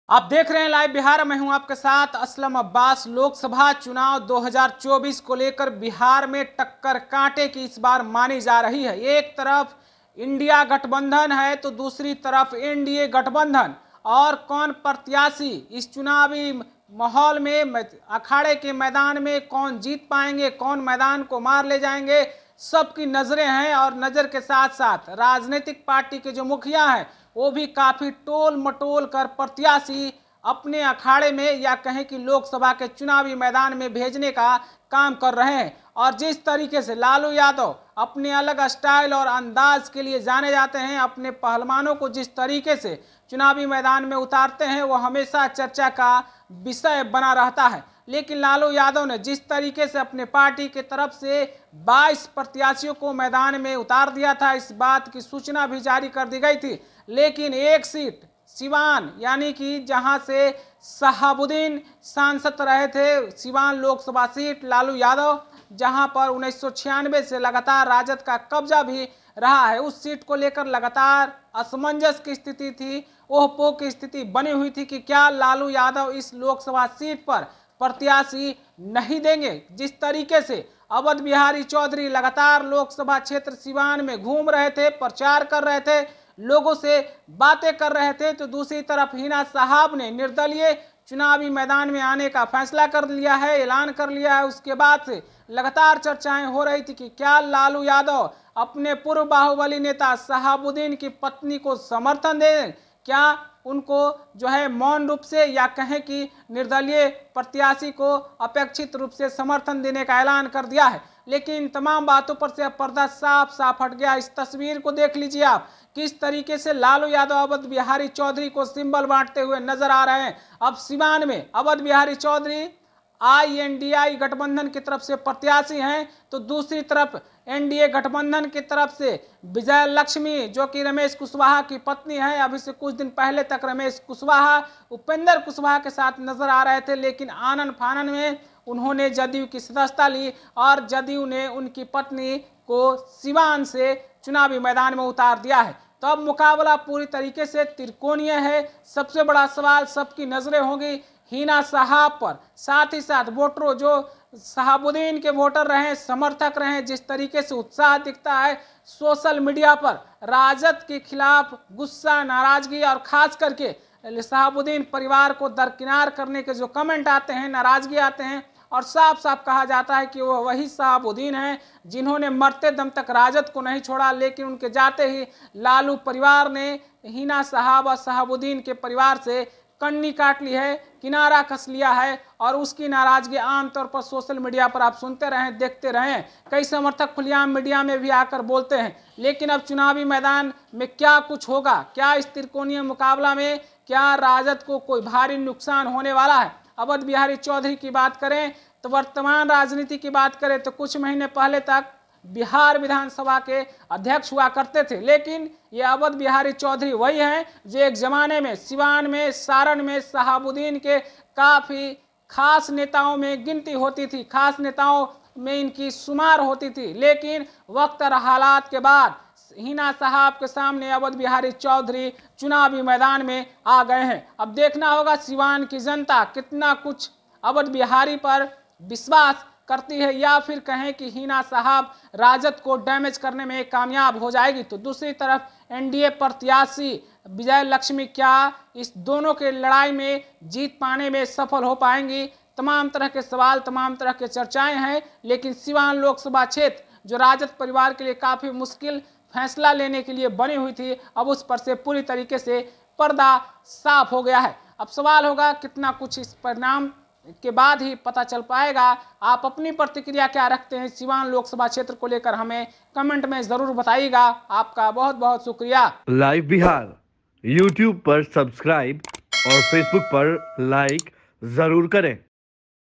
Siwan Loksabha में राजद ने सस्पेंस खत्म कर दिया, Heena Shahab के खिलाफ Awadh Bihari को टिकट..NDA से
Live Bihar Digital Network
Siwan Loksabha में राजद ने सस्पेंस खत्म कर दिया, Heena Shahab के खिलाफ Awadh Bihari को टिकट..NDA से तो...